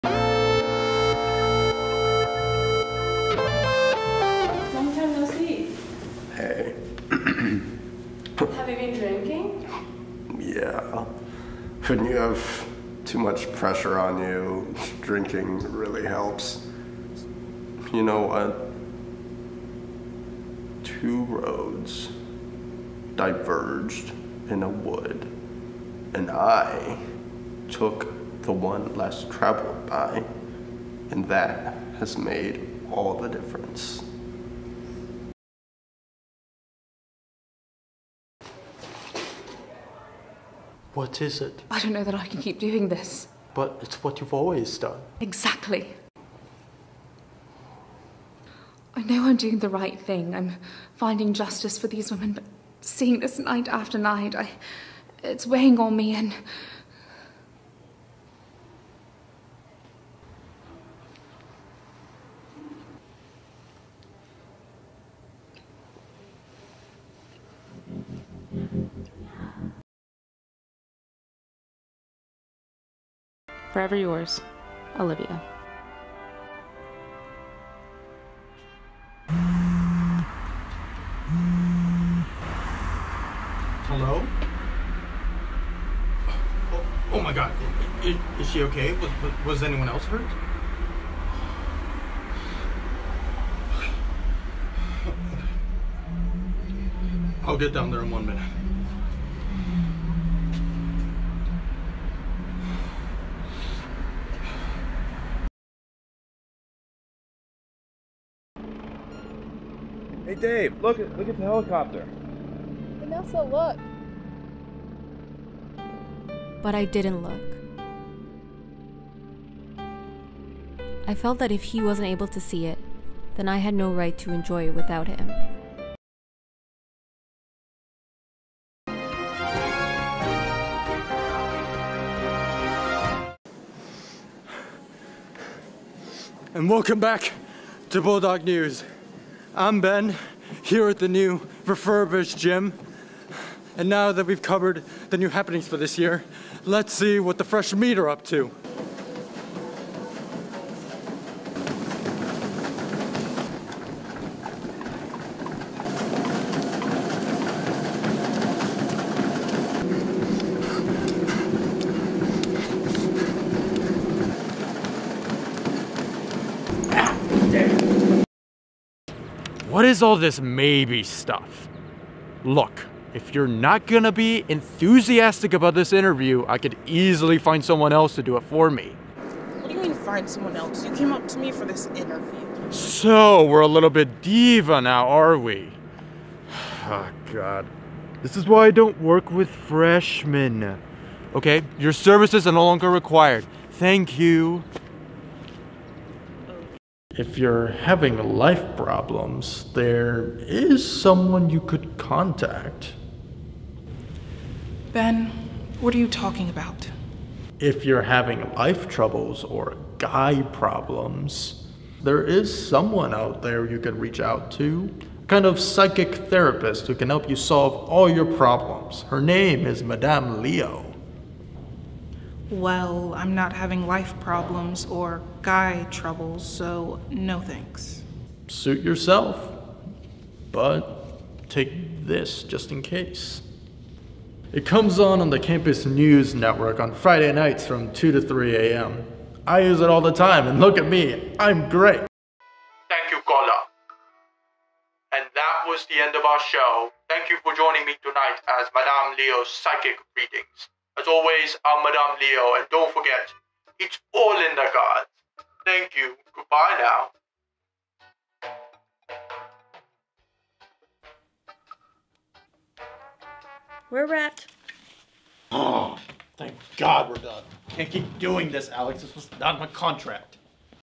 Hugo Hernandez Villagra: Acting Reel
From comedy to drama, I am excited to showcase my acting reel.